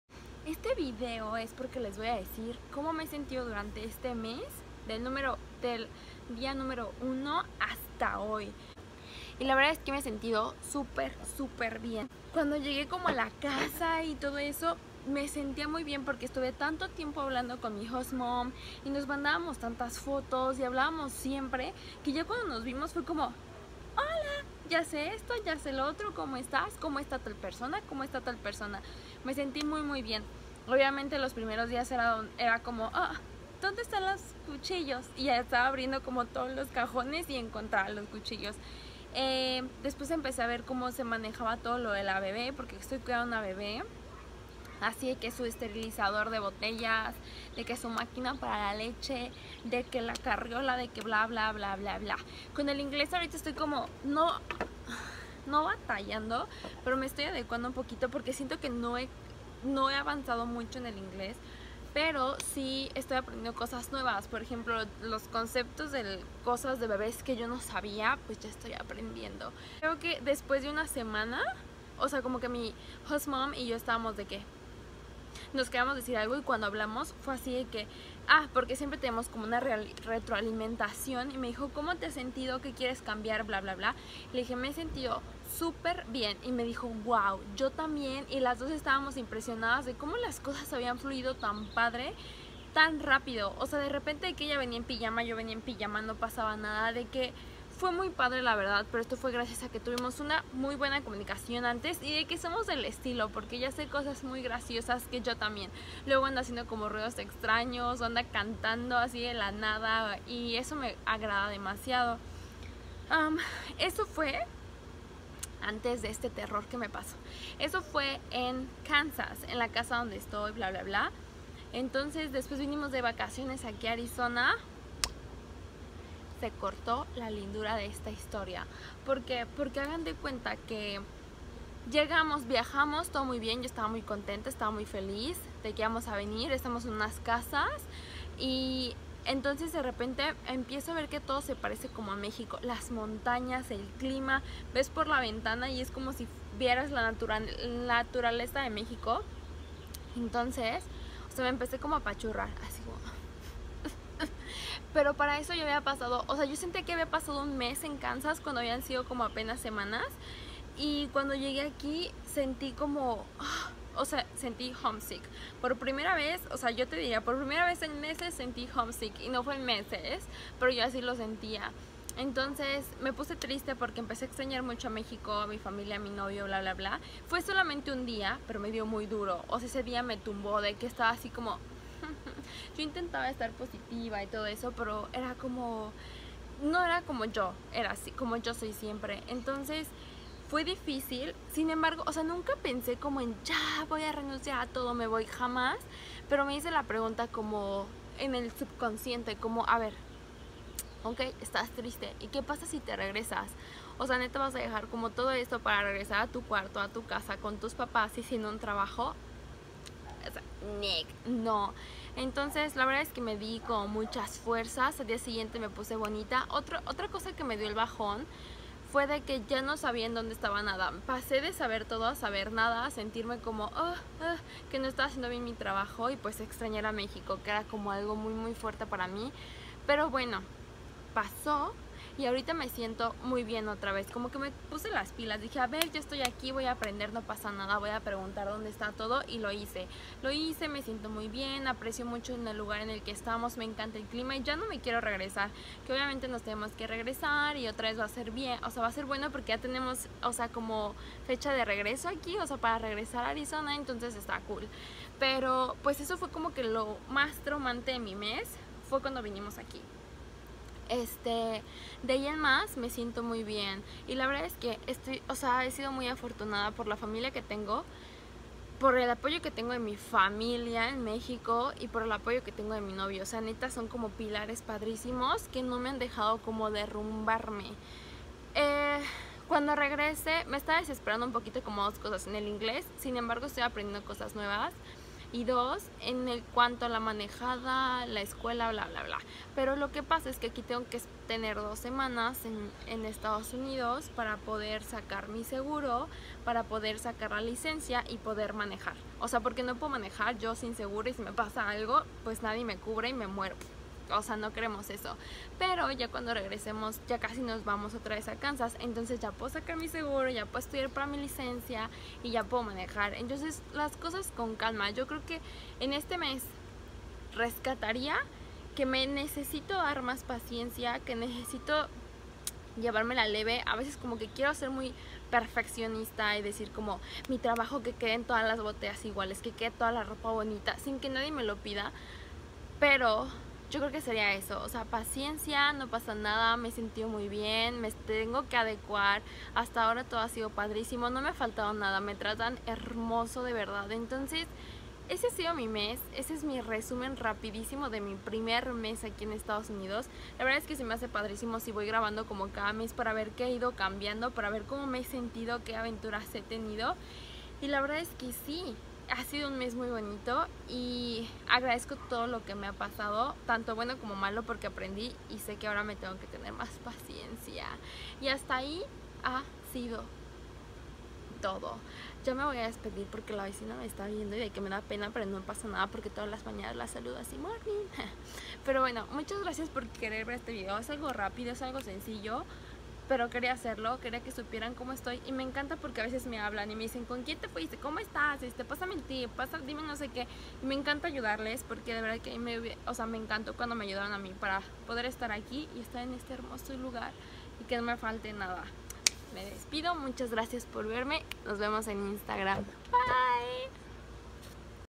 Primer Mes Siendo Au Pair: El Peor Homesick
Amigos! En este video les platico como fue mi primer mes siendo Au Pair y aunque todo fue muy positivo, con buena vibra y mucho amor por parte de mi Host Family, me dio mi primero Homesick.
Fue algo demasiado fuerte, sin embargo es parte de la experiencia de ser Au Pair.
Espero que les sirva mucho mi video y cualquier cosa saben que me pueden encontrar en insta como...